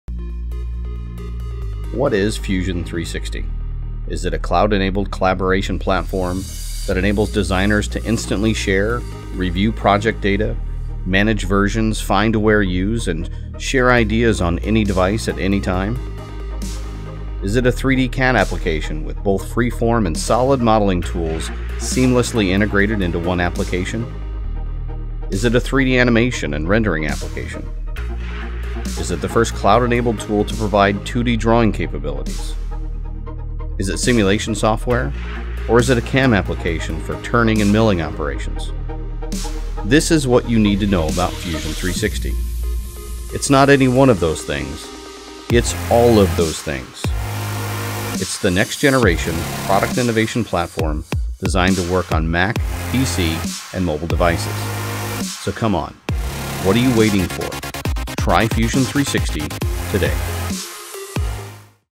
What is Fusion 360?
What is Fusion 360? Is it CAD? Is it Freeform & Surfacing? Is it Data Management? Is it Simulation Software? Or is it a 2.5 + 3 axis CAM software application?

It’s not any one of those things, it’s all of those things. And you need to check it out.